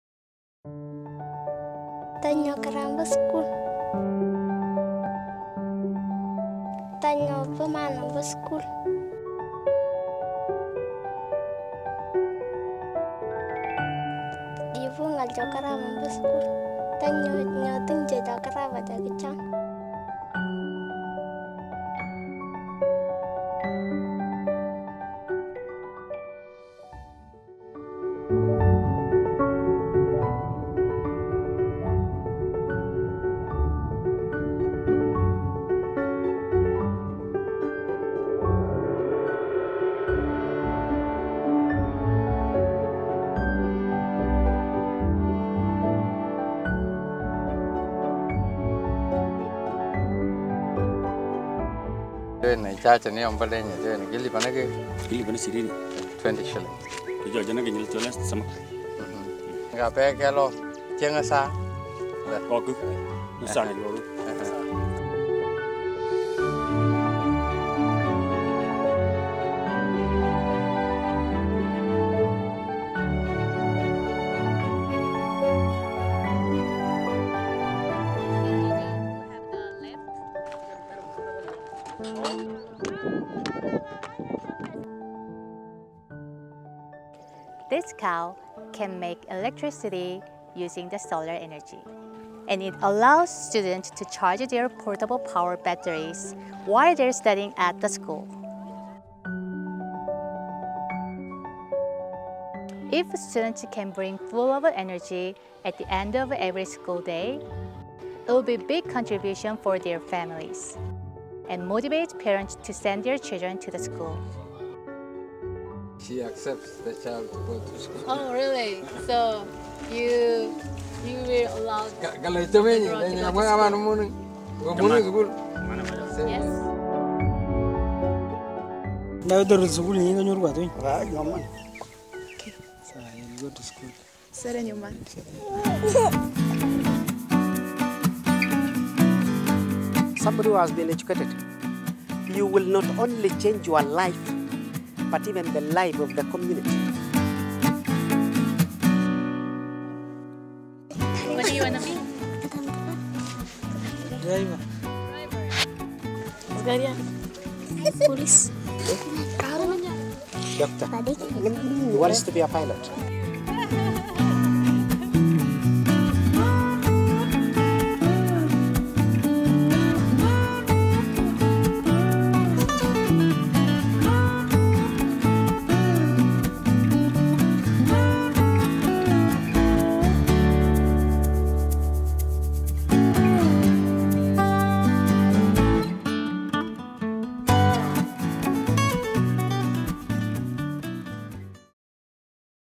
[YOLK] Solar Cow Project helps to stop child labor in Africa
The problems of child labor, education, and energy infrastructure are complex and widespread in developing nations. Usually each problem is addressed separately. However, YOLK recognizes that these three problems are deeply intertwined.  By creating the Solar Cow project, YOLK is able to chip away at each of these problems simultaneously. YOLK believes that we don't have to be a large organization with deep pockets to affect change.  Creative solutions based on love and dedication can result in real change. But, we need your help. Let’s do it together.

네이버블로그 (NAVER Blog)